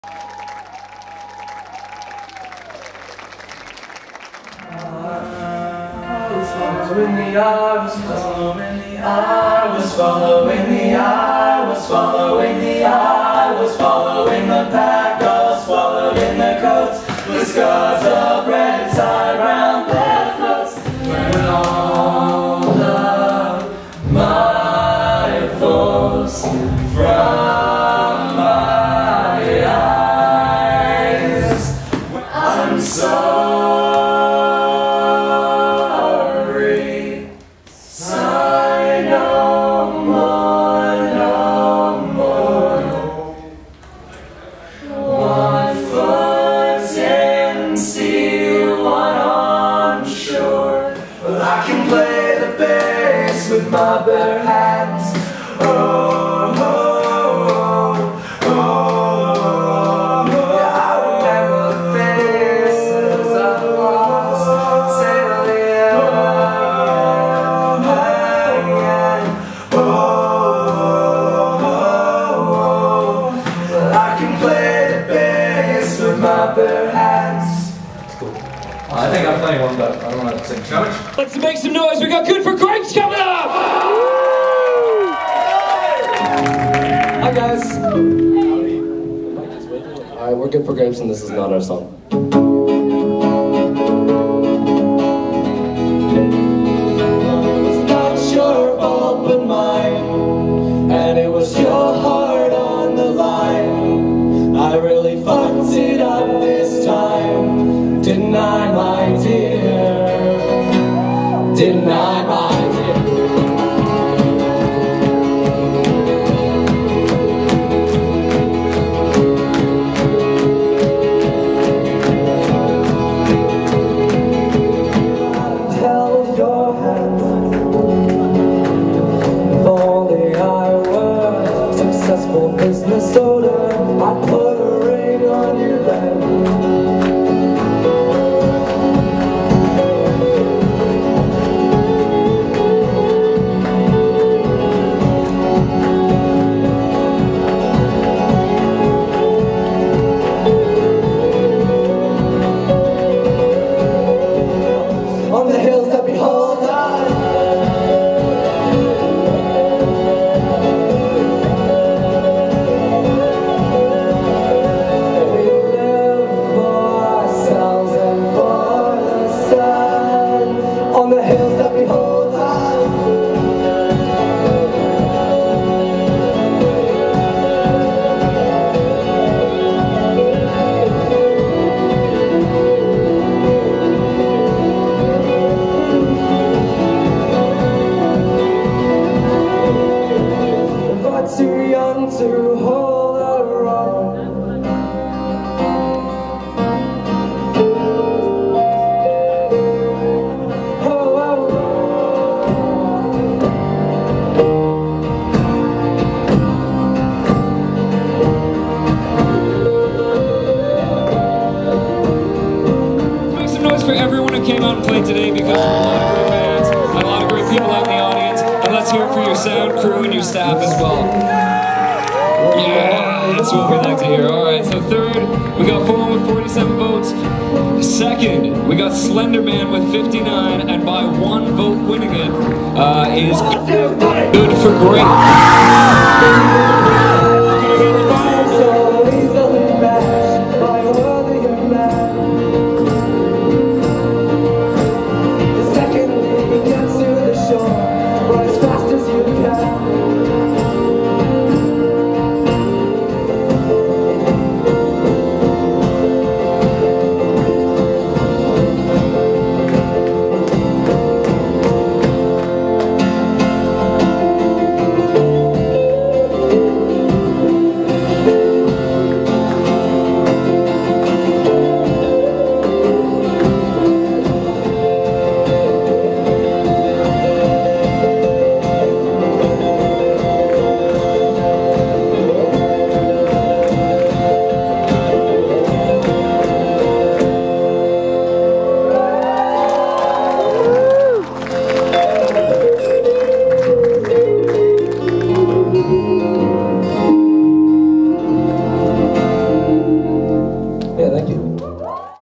Good For Grapes winning SuperNova
Just a shorter version of the other SuperNova video.

Some behind the scenes warm-ups were added so check those out.